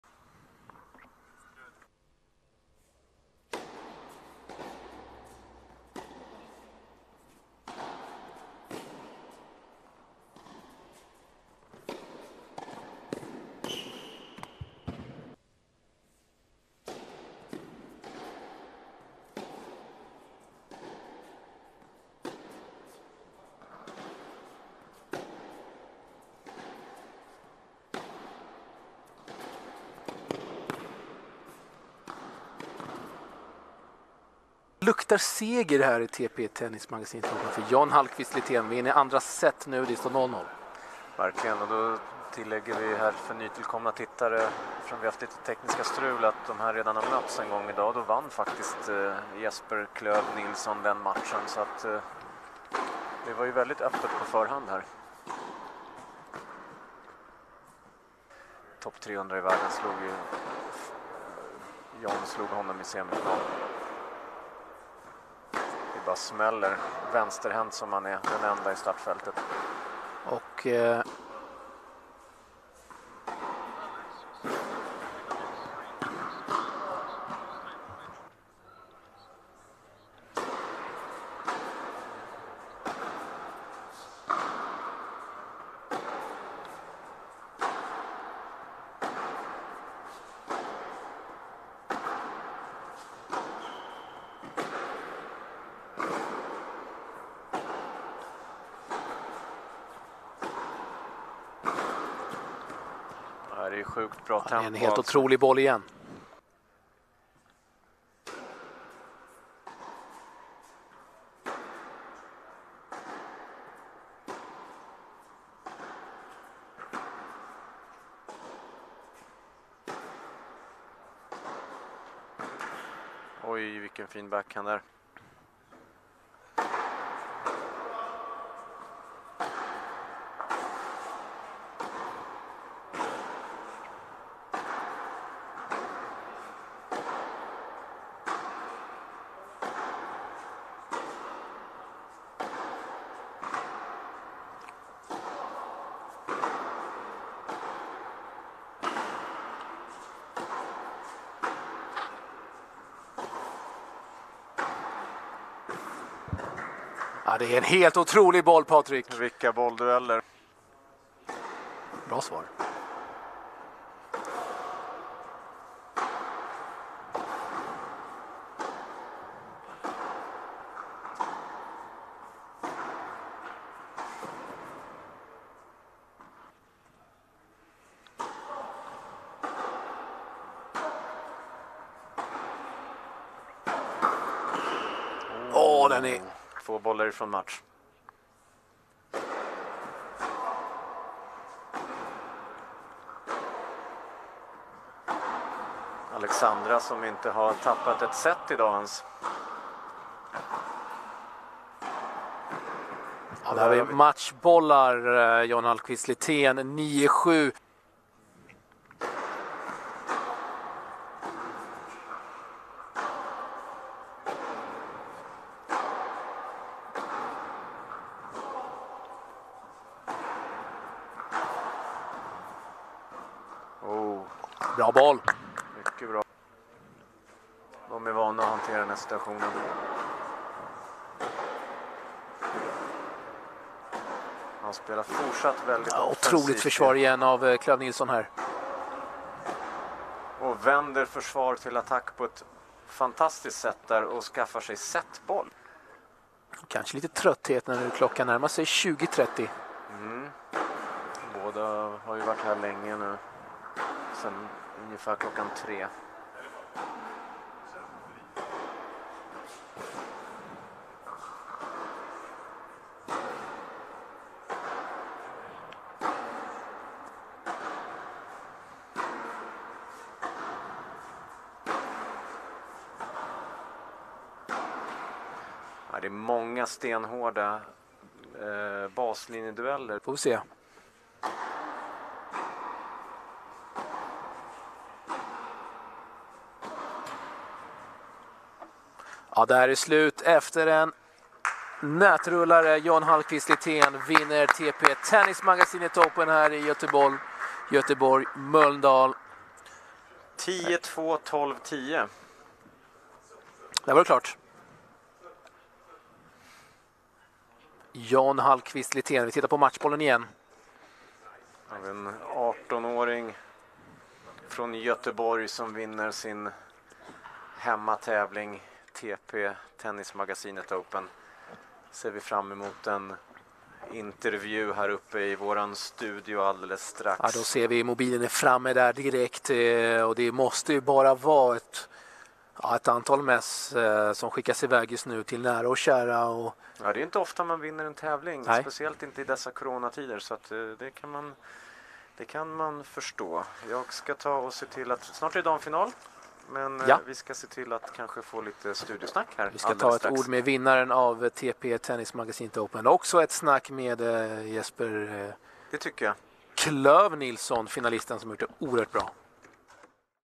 John Hallquist Lithén - Jesper Klöv Nilsson, TP/Tennismagasinet Open (Final) - Highlights
John Hallquist Lithén - Jesper Klöv Nilsson, TP/Tennismagasinet Open 2020 (Final) - Highlights

Format: Supertiebreaks, bäst av 3